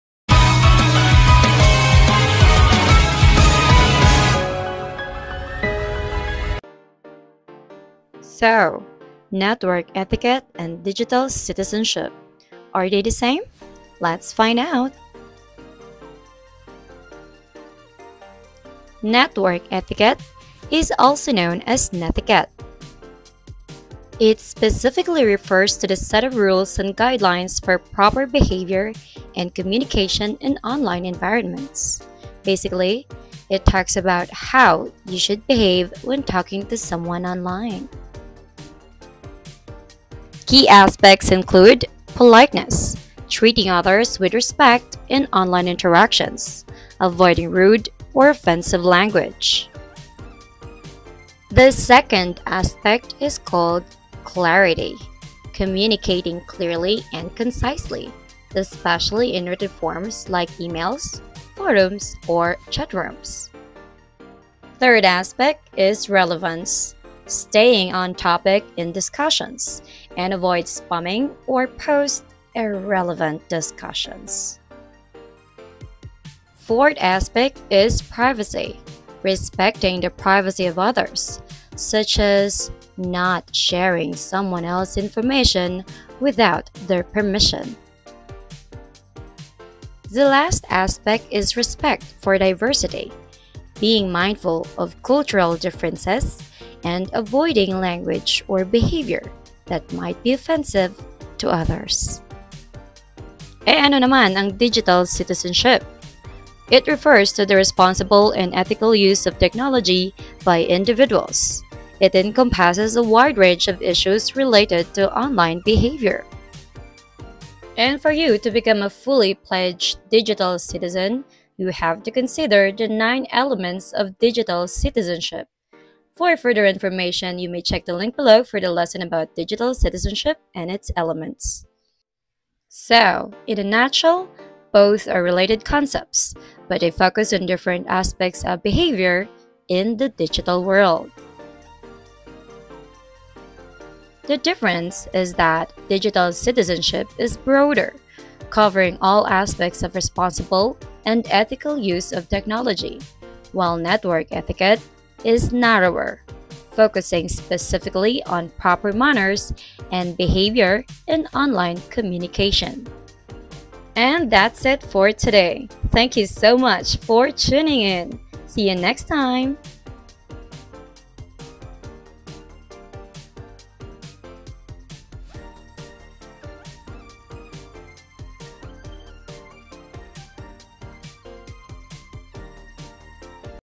Network Etiquette vs Digital Citizenship
Tackles about what is Network Etiquette and its similarities and differences to Digital Citizenship

Link on the discussion about Digital Citizenship and Its Elements